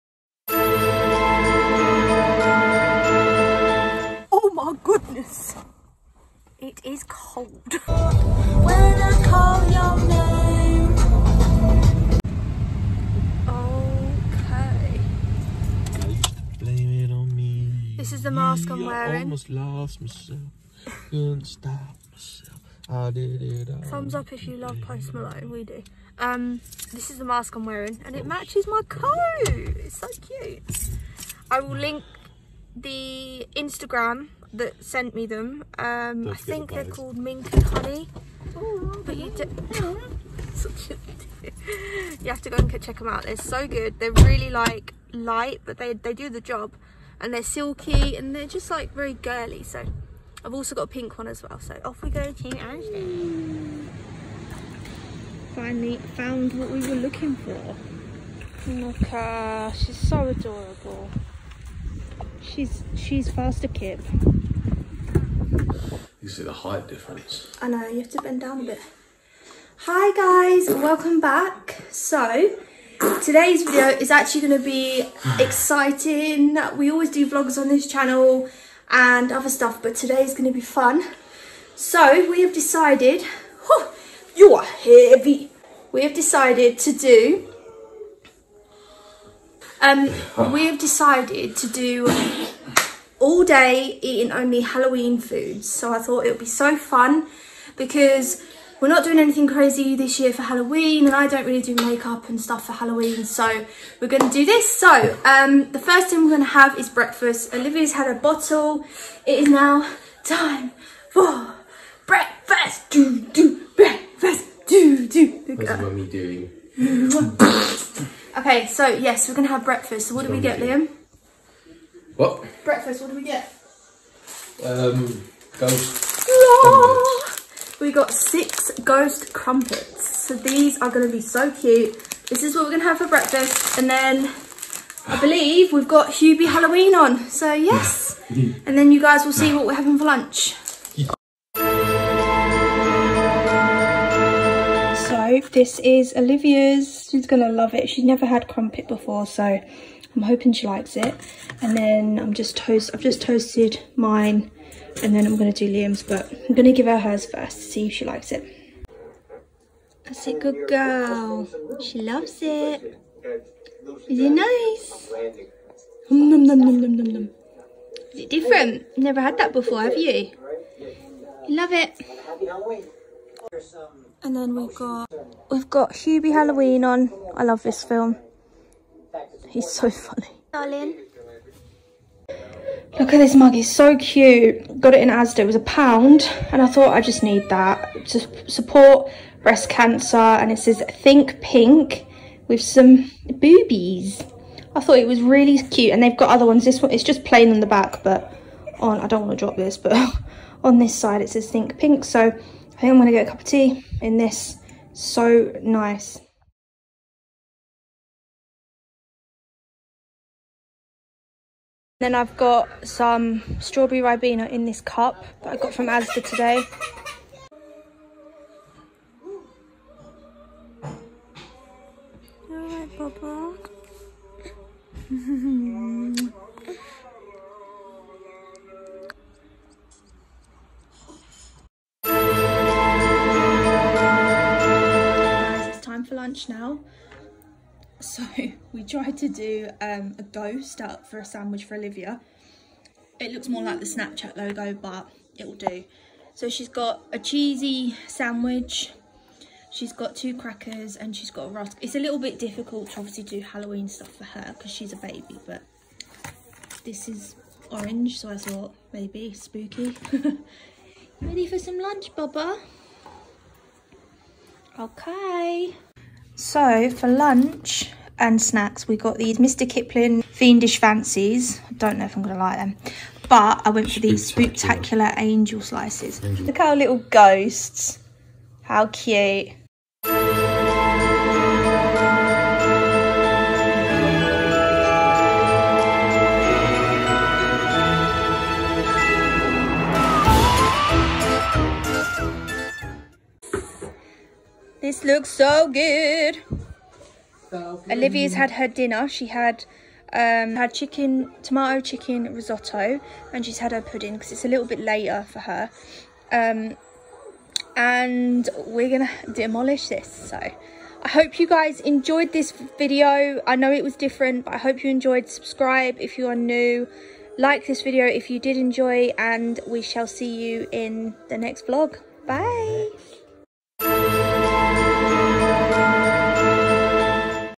Eating ONLY HALLOWEEN Food For 24 Hours!! 🎃👻
We only eat halloween foods for 24 hours. We have a 10 month baby so it was a challenge haha! I hope you enjoy. ❤️❤️ happy halloween!! 👻🎃

Are you a digital influencer/ blogger, join my private Facebook group